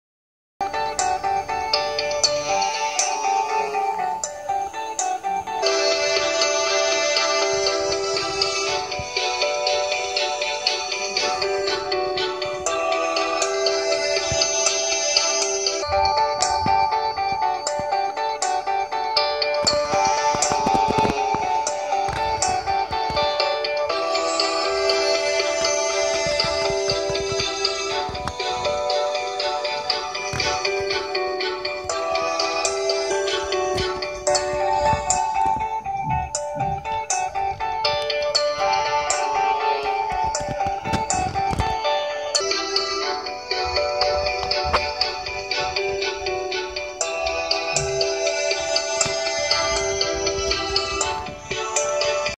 Homemade hologram
You need: cutted CD covers, tablet (smartphone), holographic movie